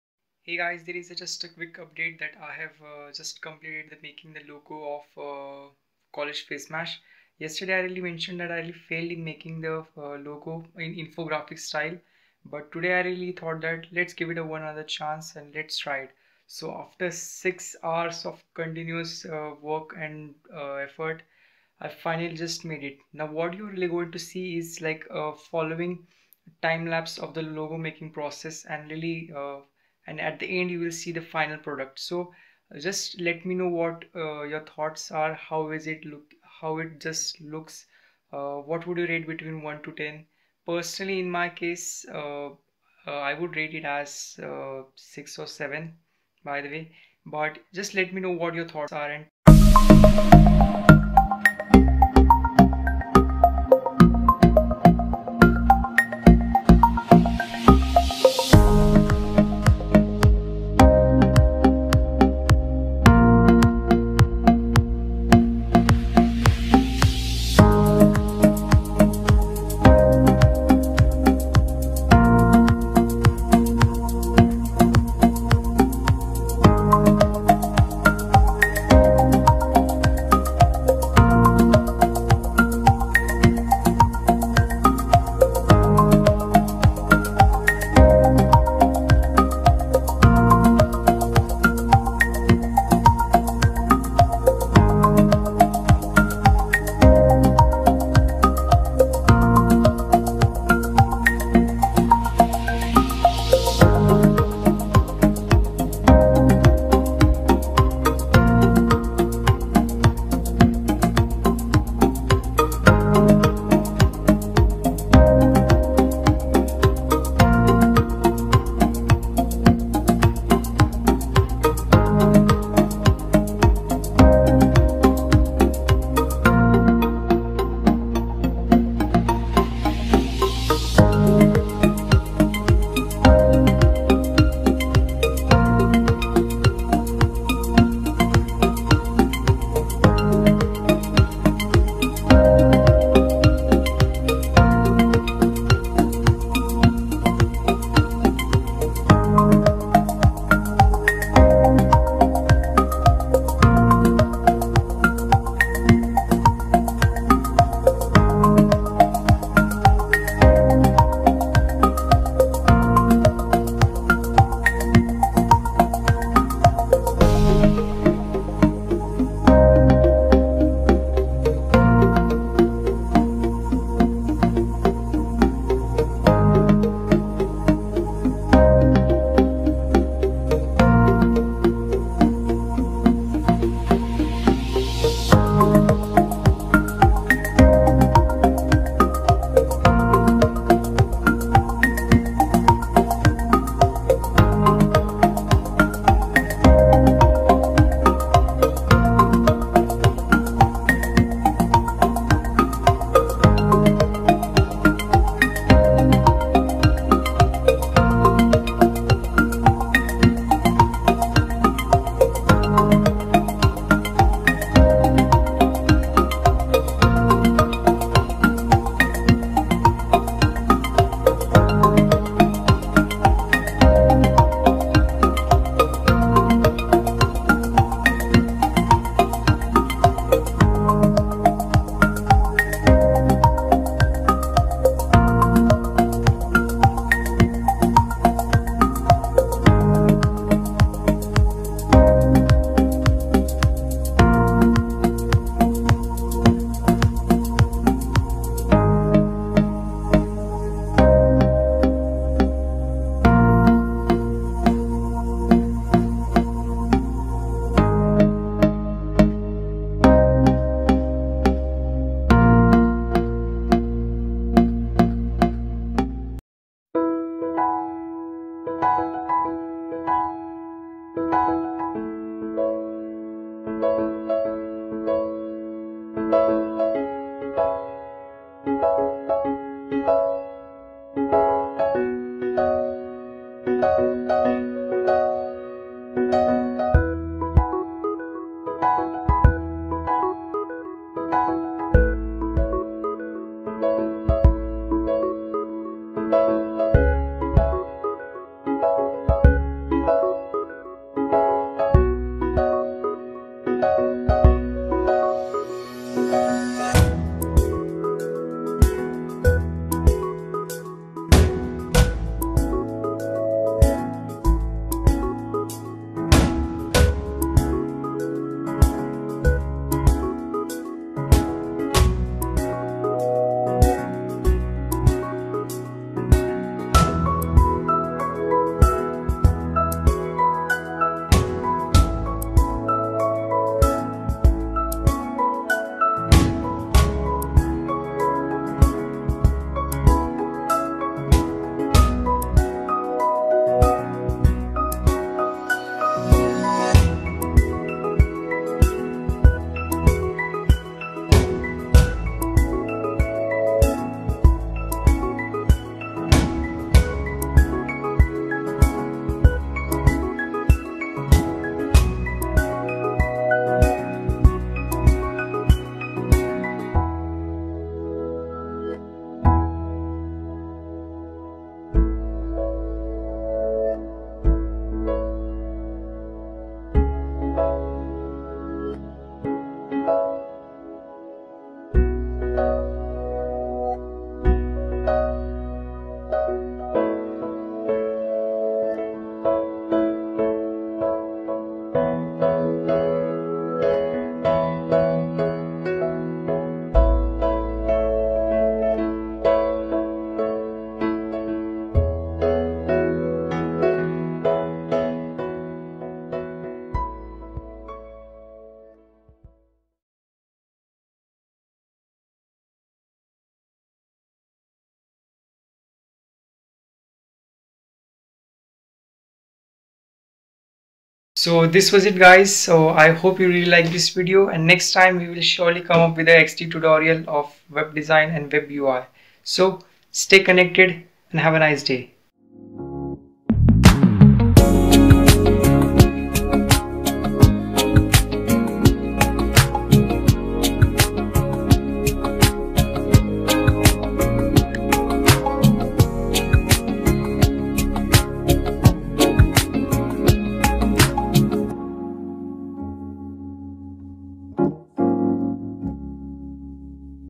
I tried again and finally made a logo| How to make Facemash
In this video i have shown how i finally made the infographic logo for facemash.

If you have any suggestions, Queries or any though just leave it in comment and I'll be happy to get back to you.